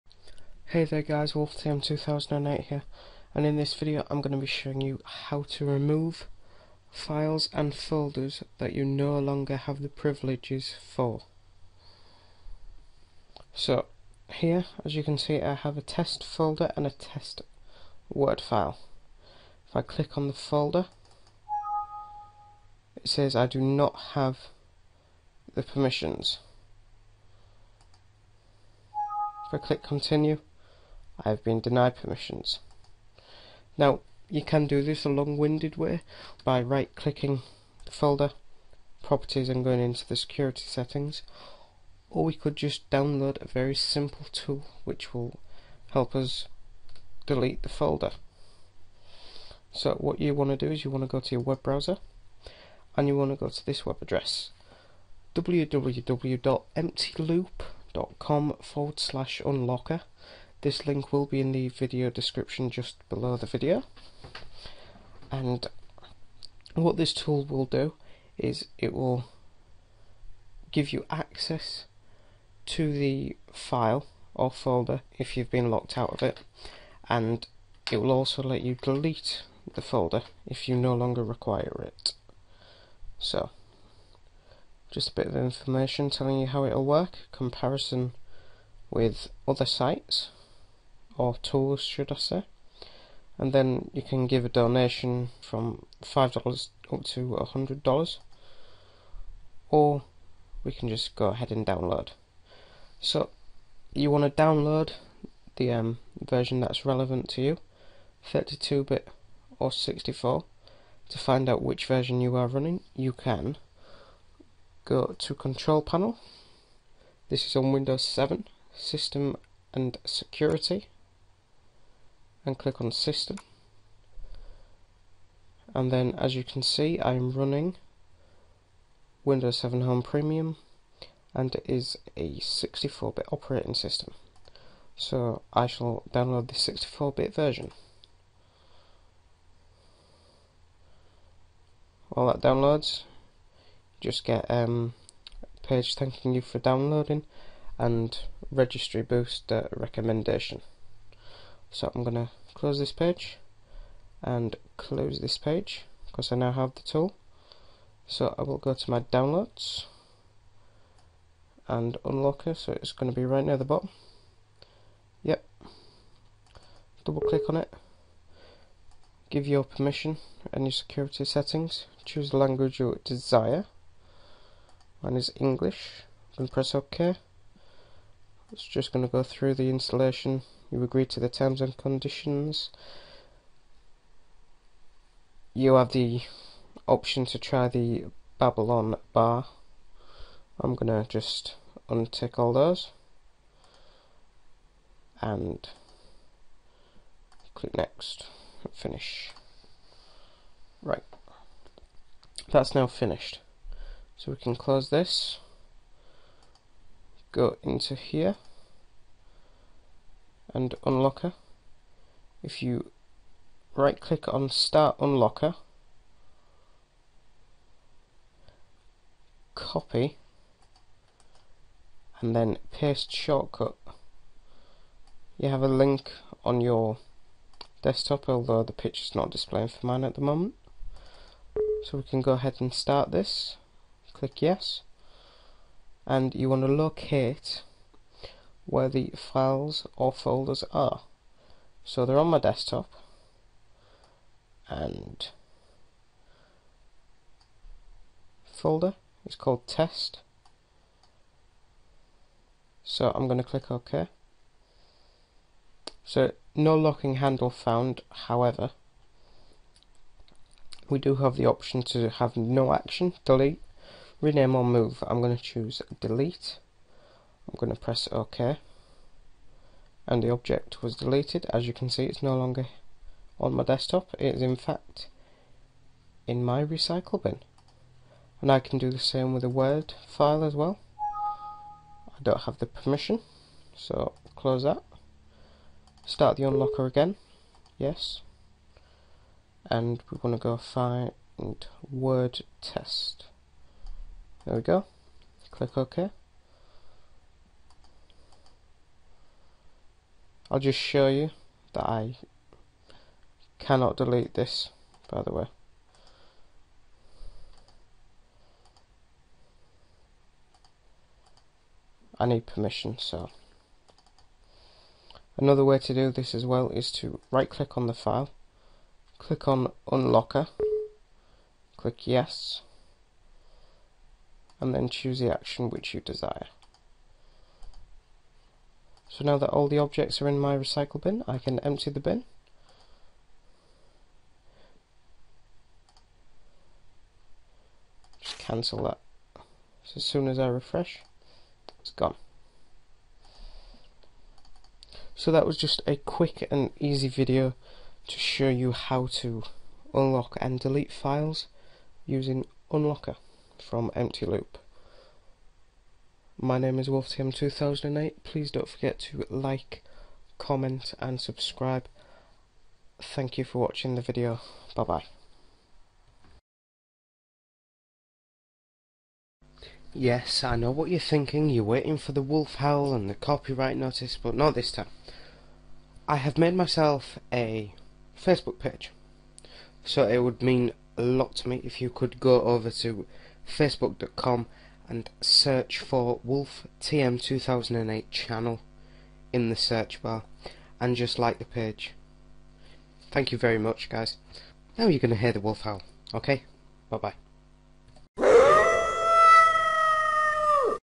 Unlock & Delete Locked Files & Folders using Unlocker (Windows Only)
PLEASE READ!!!
Hi Guys, in this video I will be showing you how you can delete files and folder that you no longer have the permissions for.